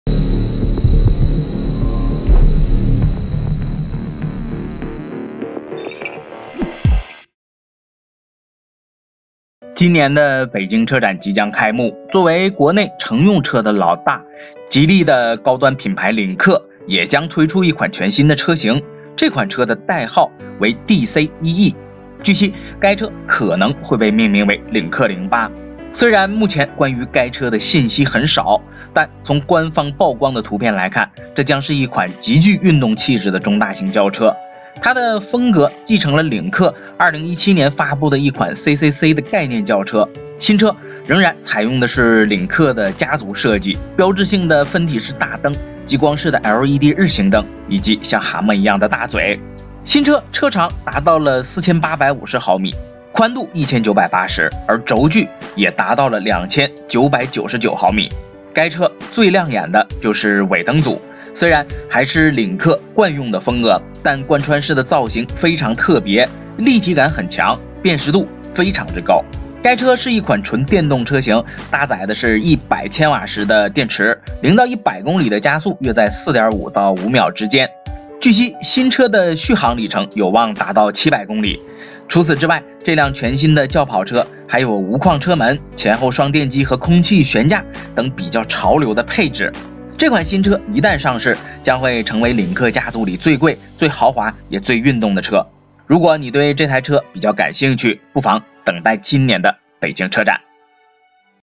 Geely Lynk&co 08 sedan | 吉利领克08将亮相，中大型轿跑轴距3米，或4.5秒破百！
今年的北京车展即将开幕
作为国内乘用车的老大
吉利的高端品牌领克也将推出一款全新的车型
这款车代号为DC1E
据悉 该车可能被命名为领克08
虽然目前关于该车的信息很少
但从官方曝光的图片来看
这将会是一台极具运动气质的中大型轿车
他的风格继承了领克2017年发布的一款ccc的概念轿车
新车仍然采用的是领克的家族设计
标志性的分体式大灯 极光式的LED日行灯
以及像蛤蟆一样的大嘴
新车的车长达到了4850毫米 宽度1980
而轴距也达到了2999毫米
该车尾部最扎眼的就是尾灯组
虽然还是领克的惯用风格
但贯穿式的造型非常特别 立体感很强
辨识度非常之高
该车是一款纯电动车型
搭载的是100千瓦时的电池
0~100公里加速约在4.5~5秒之间
据悉新车的续航里程有望达到700公里
除此之外 这辆全新的轿跑车
还有无框车门 前后双电机和空气悬架等潮流配置
这款车一旦上市
将会成为领克家族里最贵最豪华也最运动的车
如果你对这台车感兴趣
不妨等待今年的北京车展